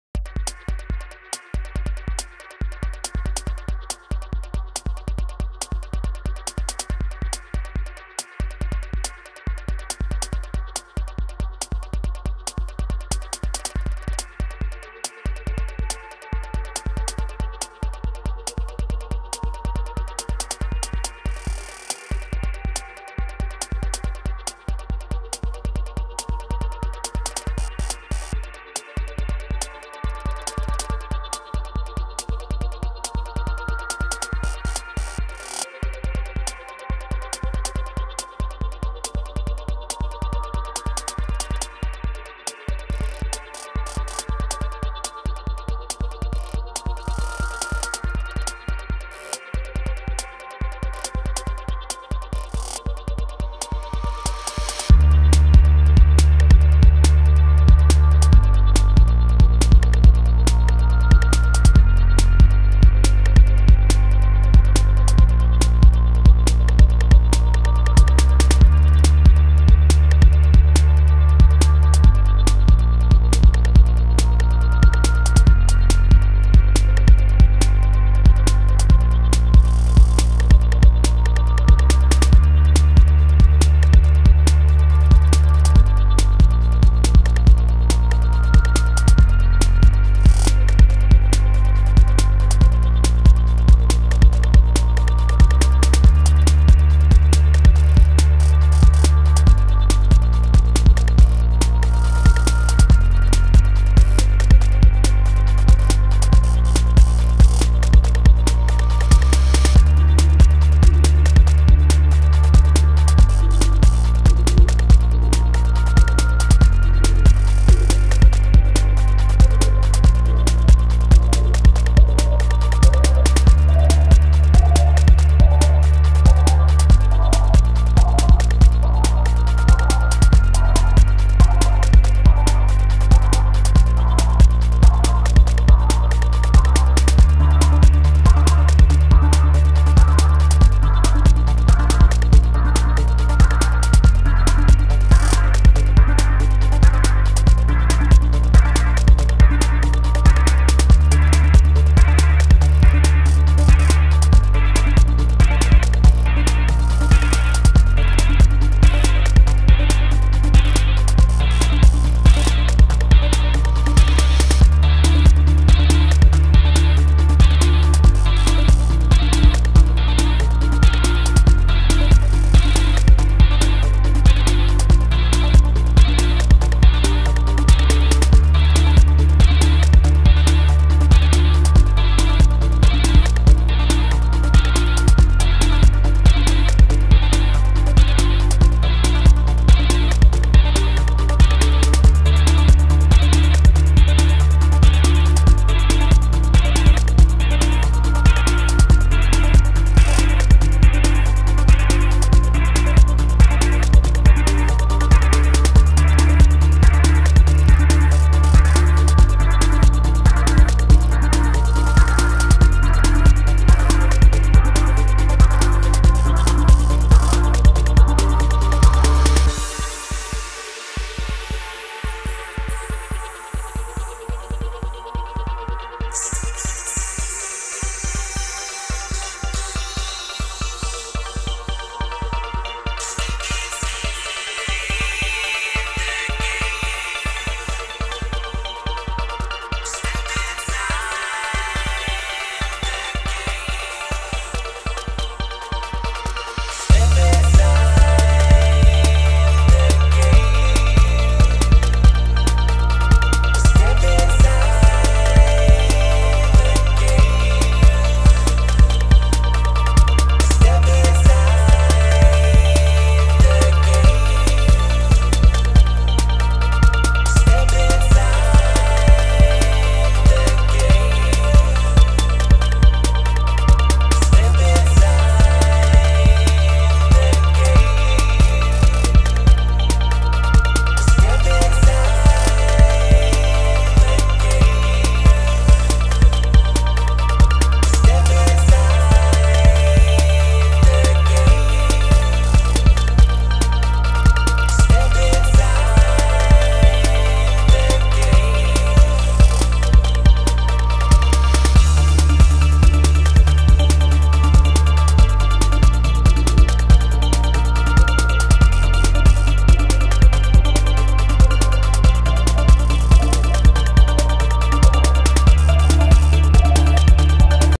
EYECANDY: telehack screencapture ratpoison wm
quick demo of ratpoison wm, the terminals are showing telehack (join the game by typing 'telnet telehack.com' in a console window). ratpoison is a tiling window manager, completely keyboard controlled, named thus because it kills mice.
If you are more of a clicky-clicky person, you can naturally still use a mouse, as demonstrated per ultra fresh copy paste actions above,